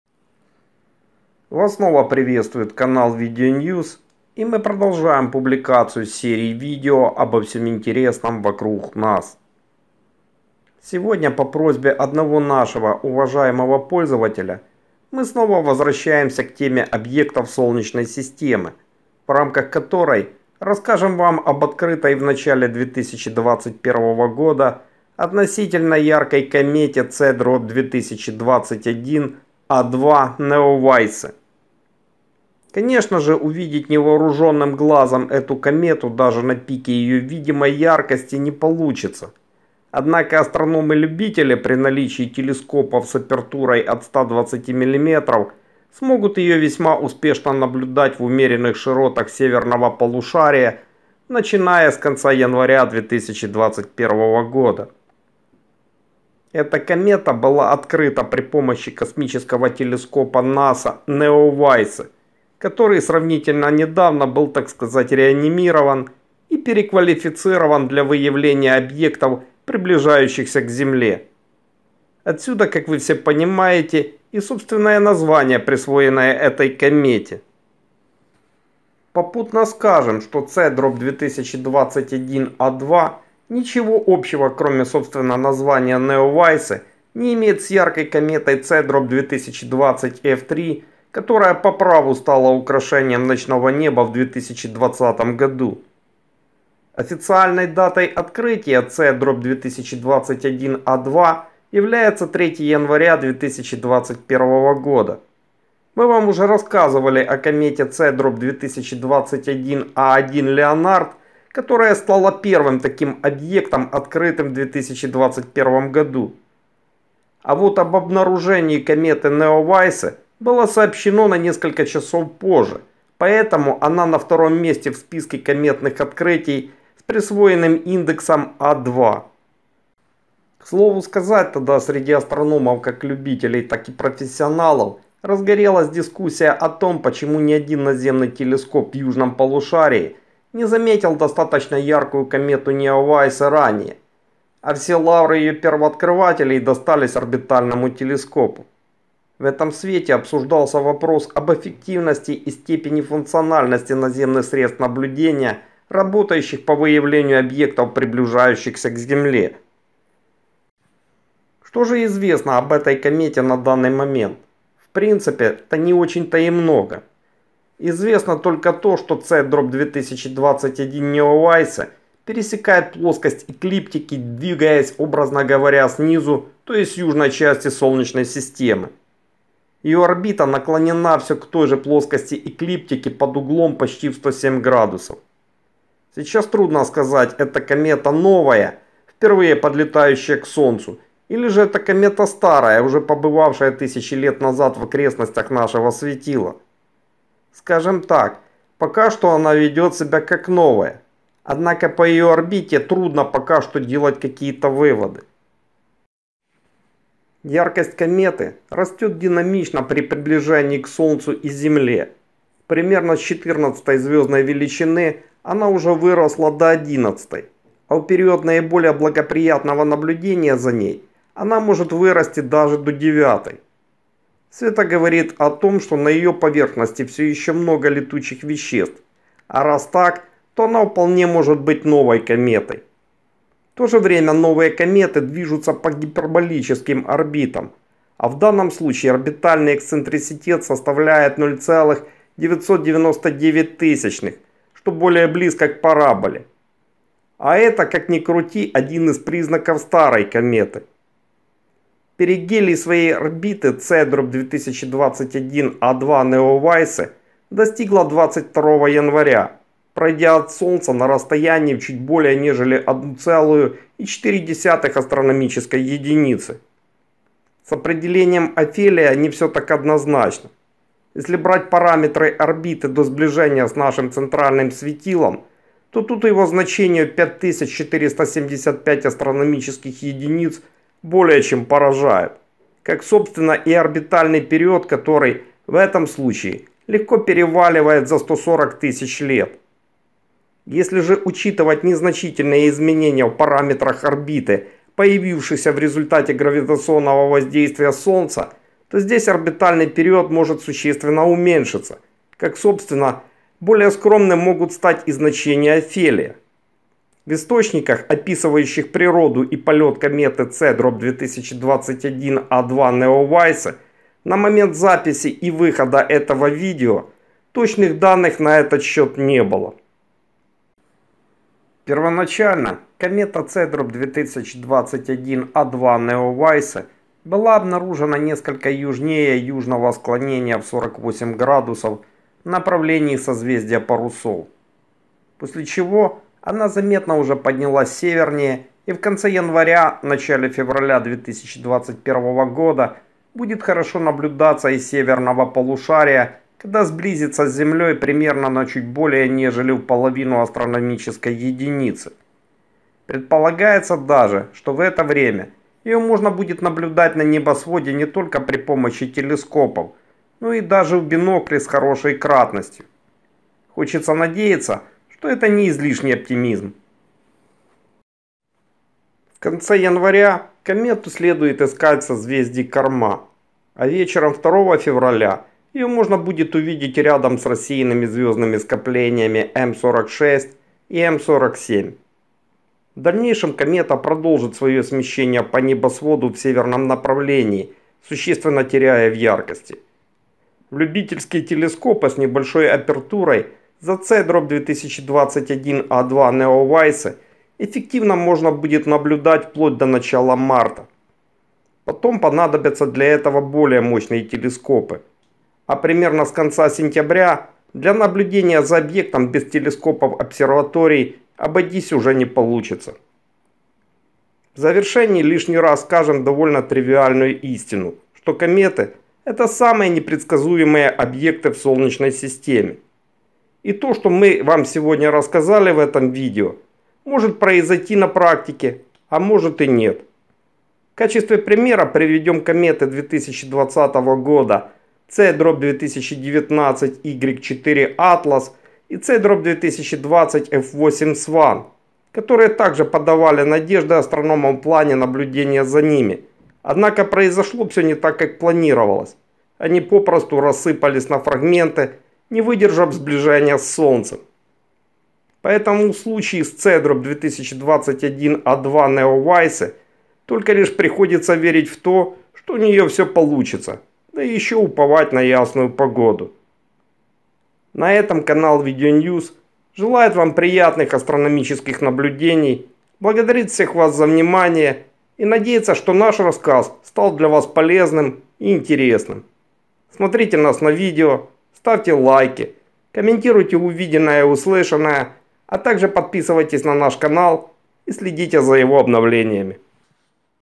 Комета C/2021 A2 (NEОWISE). Объекты Солнечной системы
Сегодня мы снова возвращаемся к теме объектов Солнечной системы, в рамках которой расскажем Вам об открытой в начале 2021 года относительно яркой комете С/2021 A2 (NEОWISE).